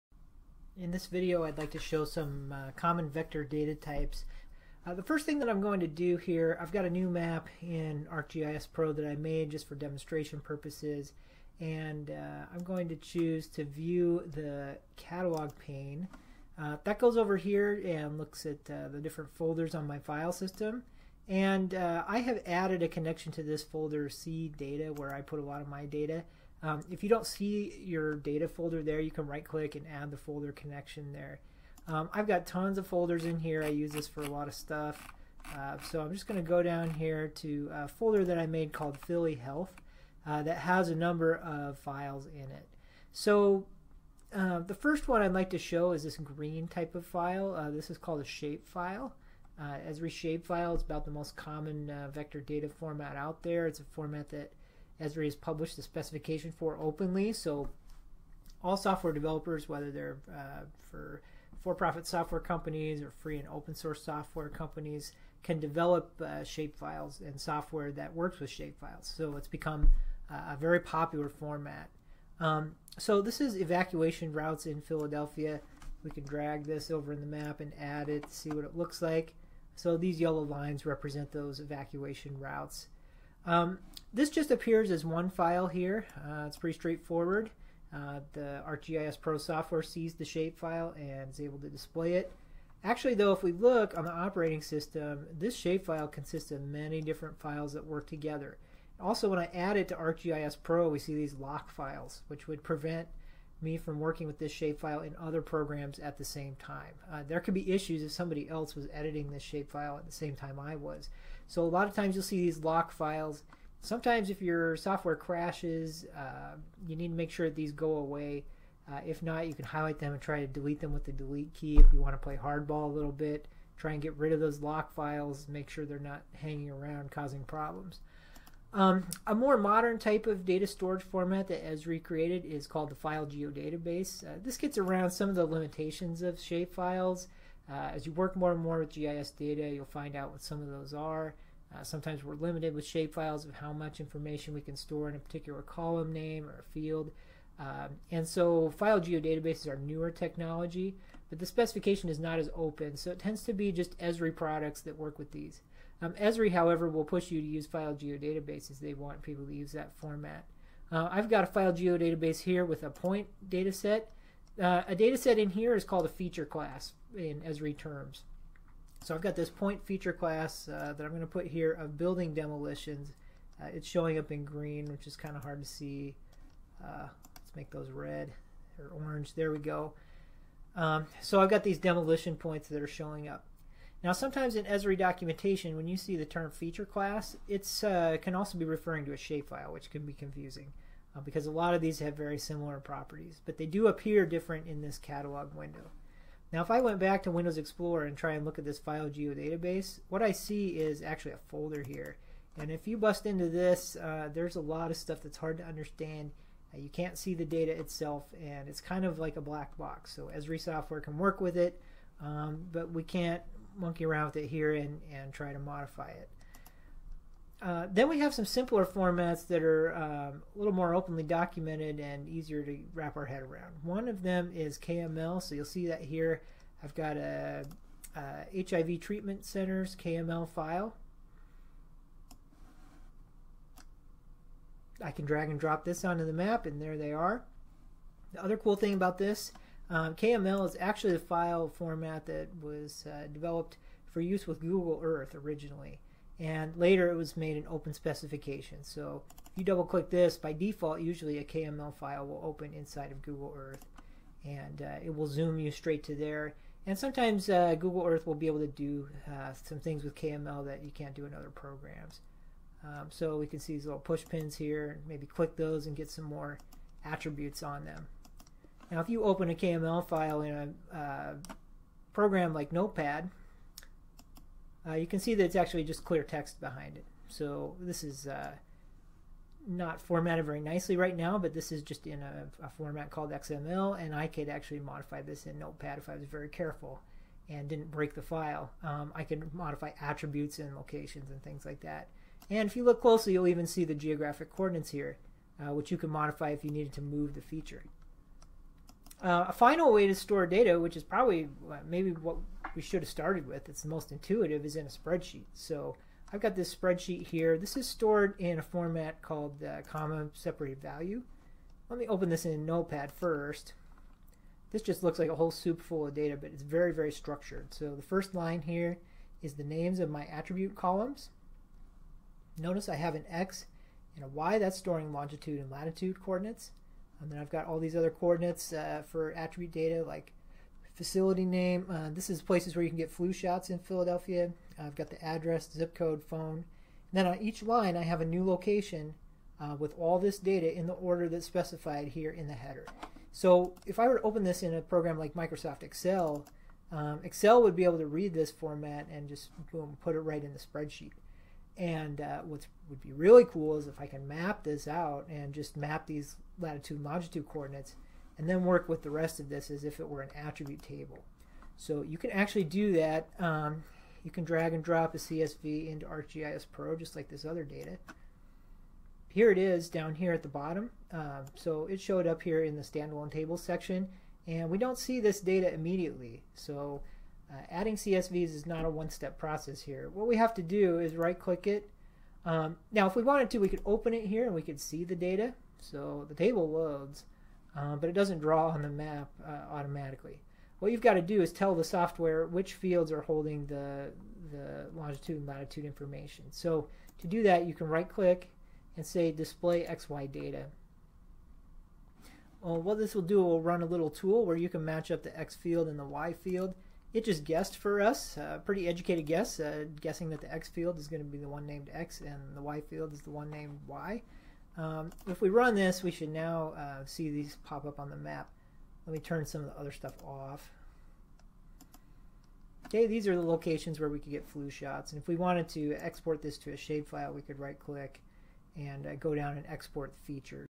Adding common vector data file formats to the map in ArcGIS Pro
Dr. Sterling Quinn demonstrates how to add shapefile, file geodatabase, KML, and CSV datasets to the map in ArcGIS Pro.

Central Washington University offers a GIS Certificate program and a Bachelor of Science degree in Geography with a GIScience specialization.